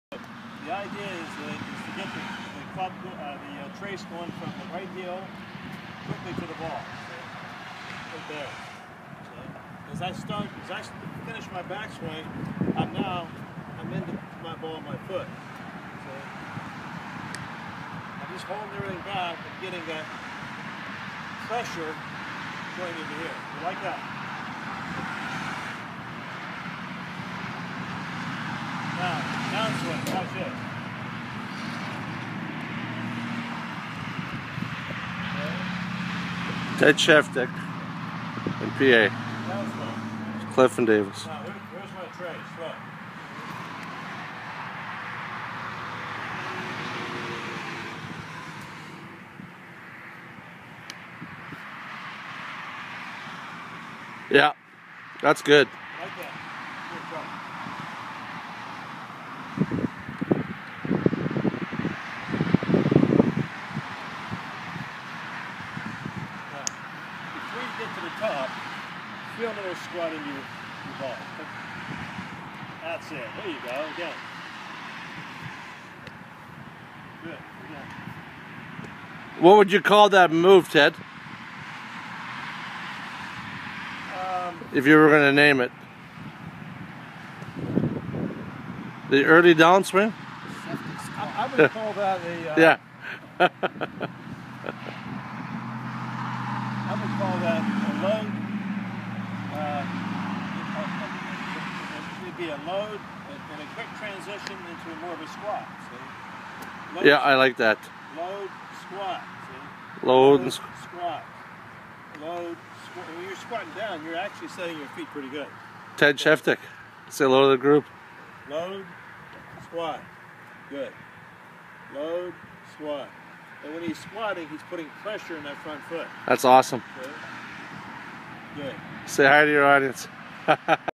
BodiTrak Sports / Ted Sheftic Golf Pressure Mapping Technique
One of the Nations Top Instructors Using Golf Pressure Mapping to Help Train his students